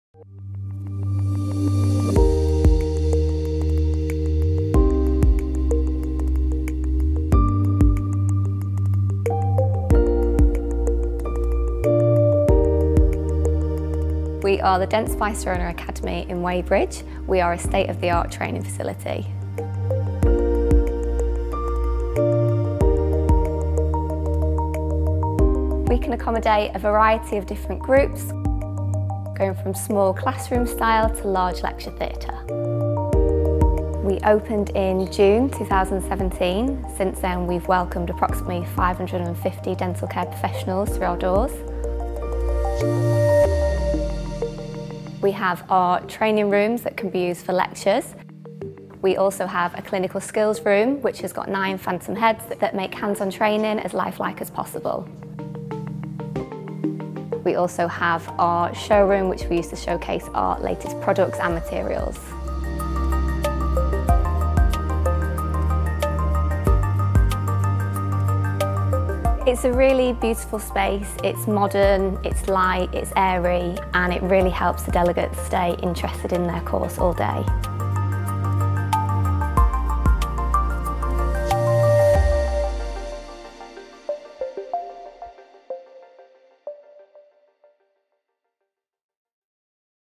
Welcome to the Dentsply Sirona Academy, London
Welkom bij de Dentsply Sirona Academy in Londen - het nieuwste state-of-the-art onderwijscentrum van Dentsply Sirona, met een 8-stations Clinical Skills Suite, een 30 en 60 zitplaatsen tellende collegezaal en een live demonstratieruimte voor de nieuwste digitale workflowtechnologieën.